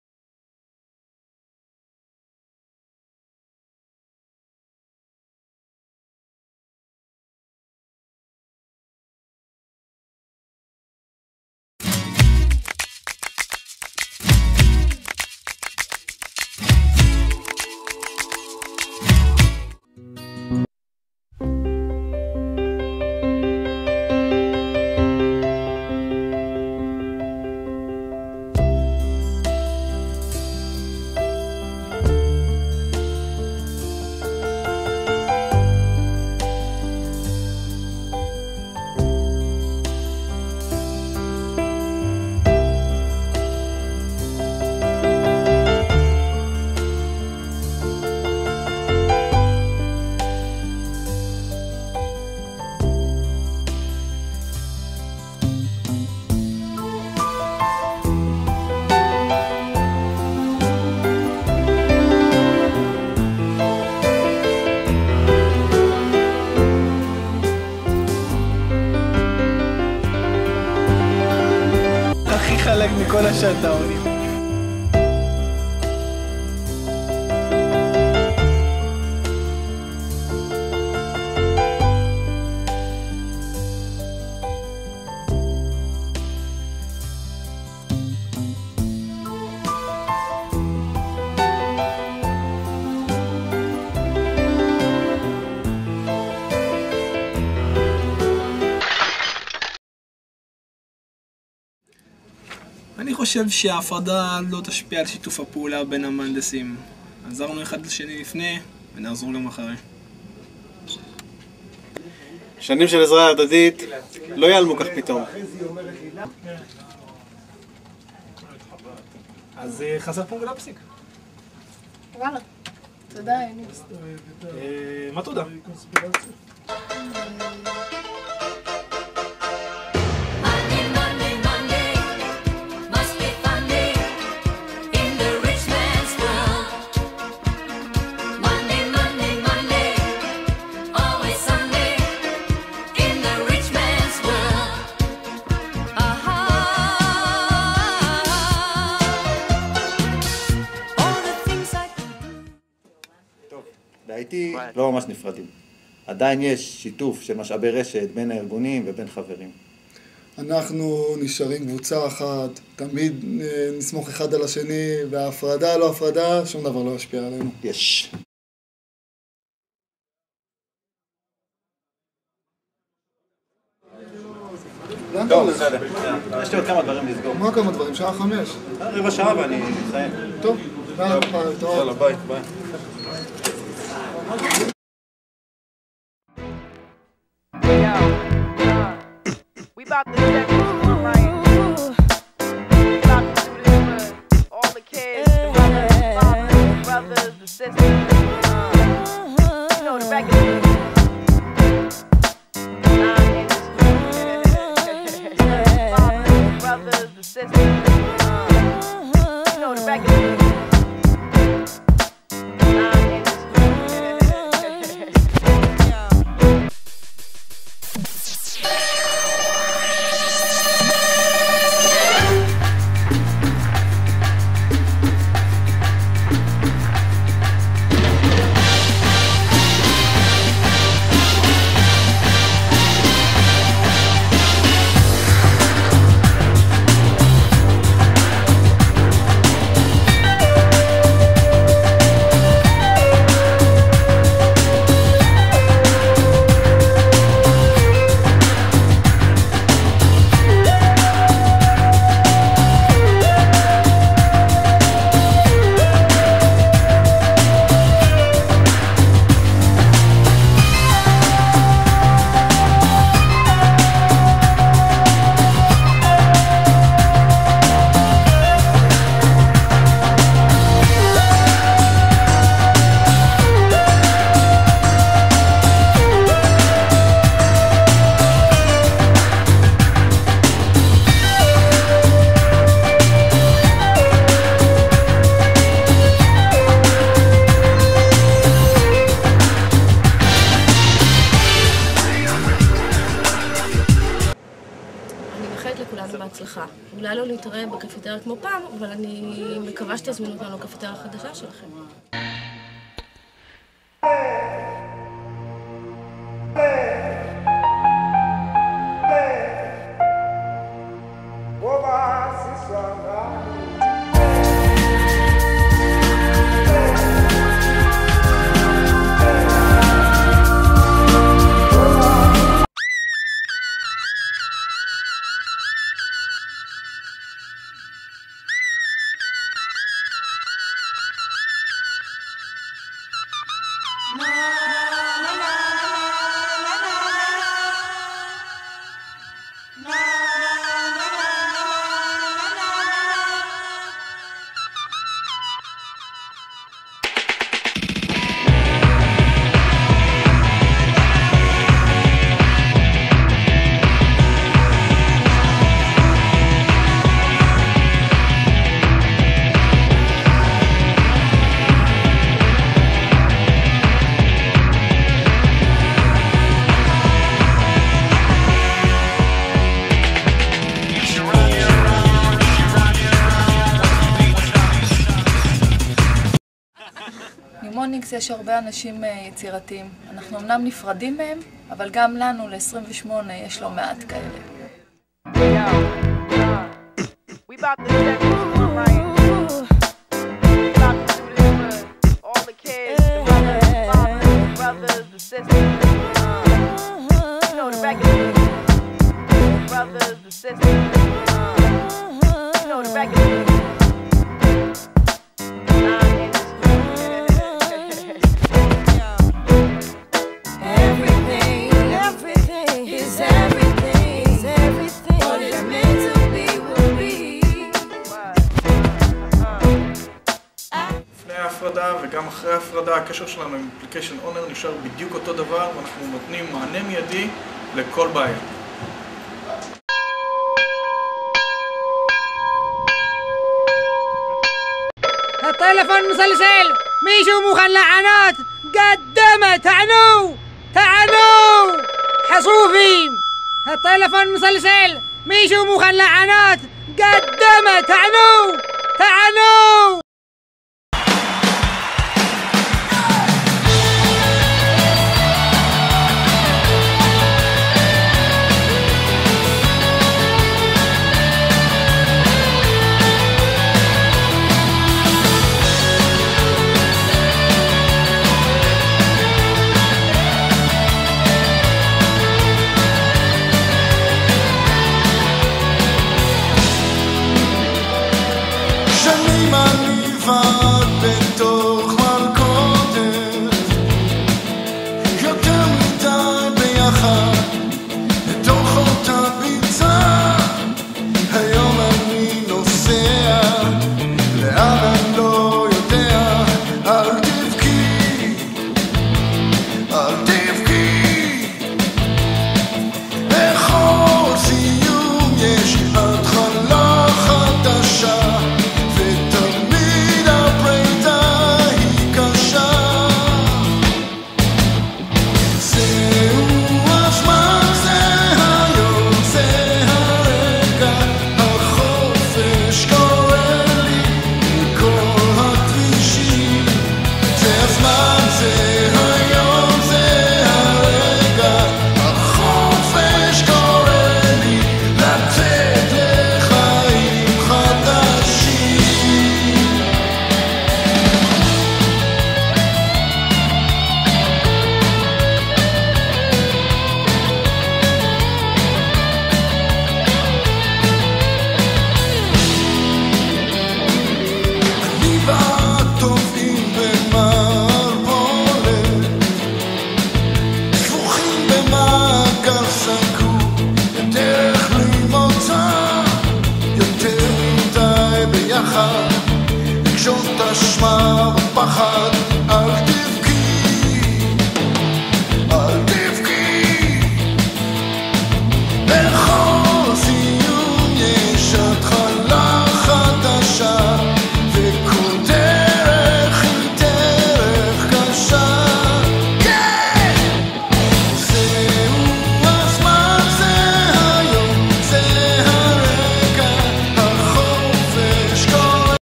אוטומציה תוכנית החלוקה - אינטל - Fab18-Fab28
סרט פרידה למחלקת אטומציה, שנת 2006 המעבר ל FAB28.